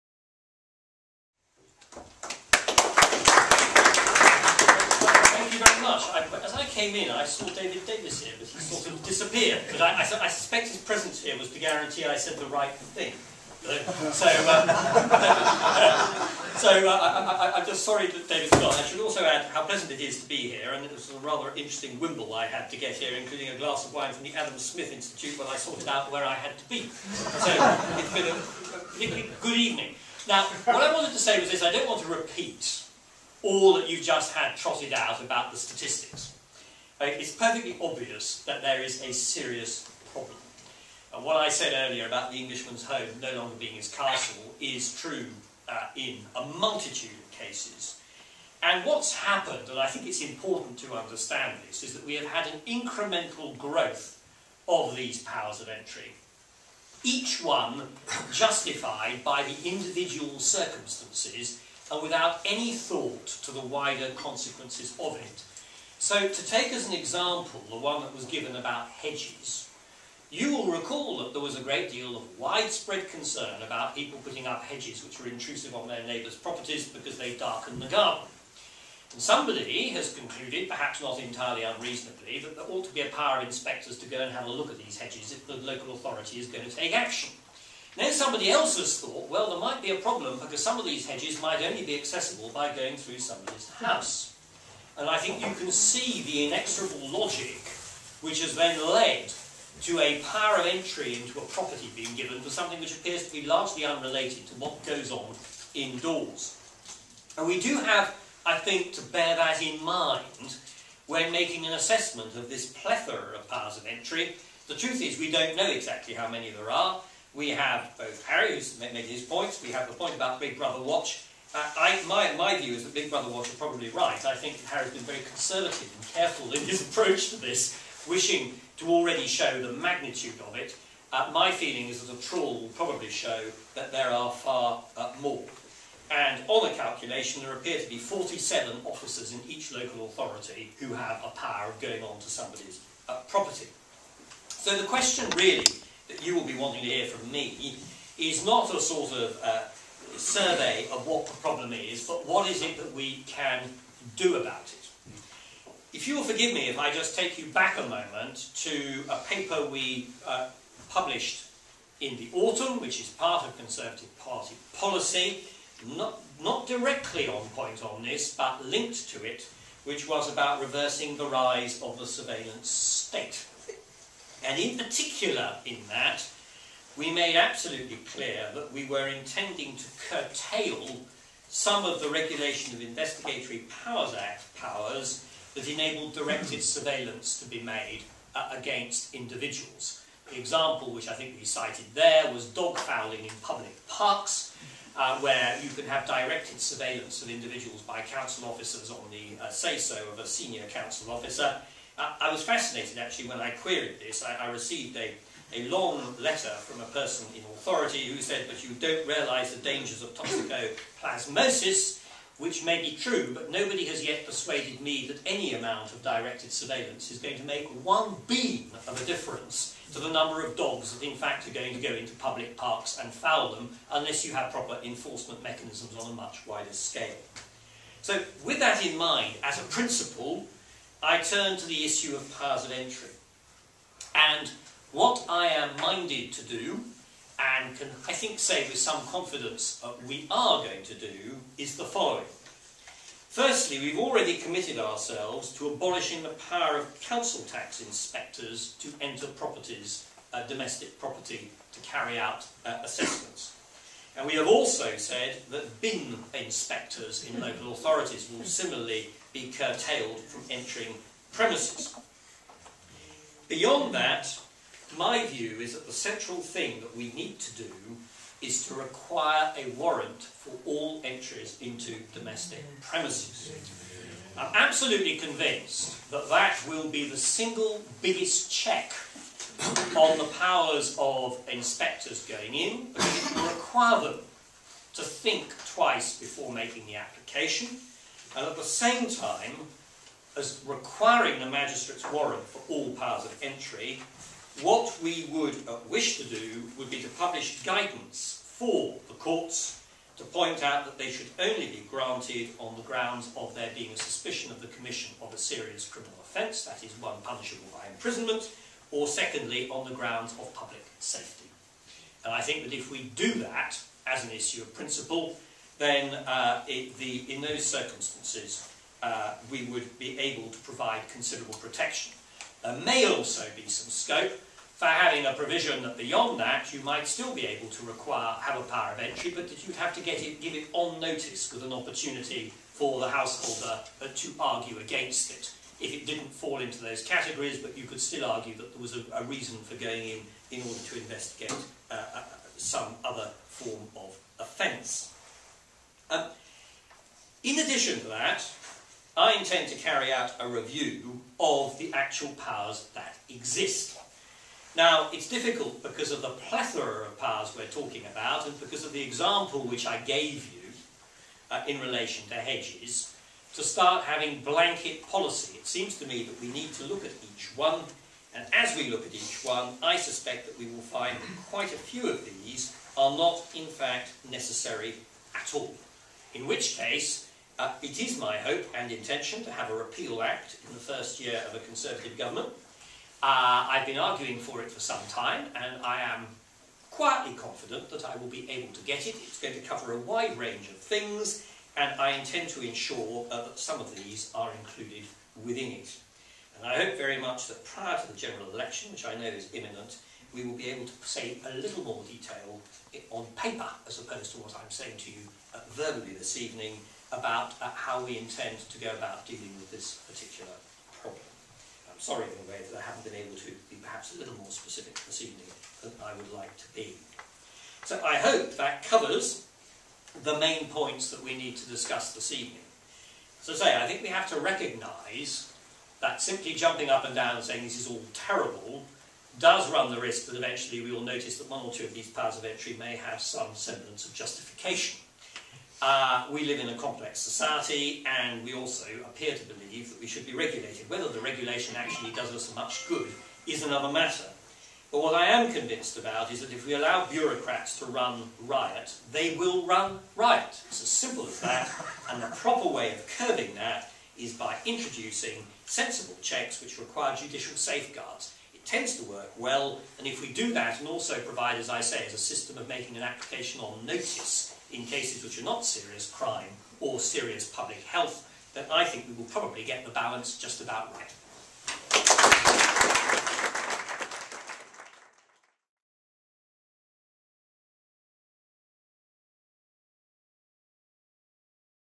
Dominic Grieve QC MP speaks at BBW/CPS event
Dominic Grieve QC MP, Shadow Minister for Justice, talks on state powers of entry at the Big Brother Watch/Centre for Policy Studies joint event - 'An Unwarranted Intrusion?'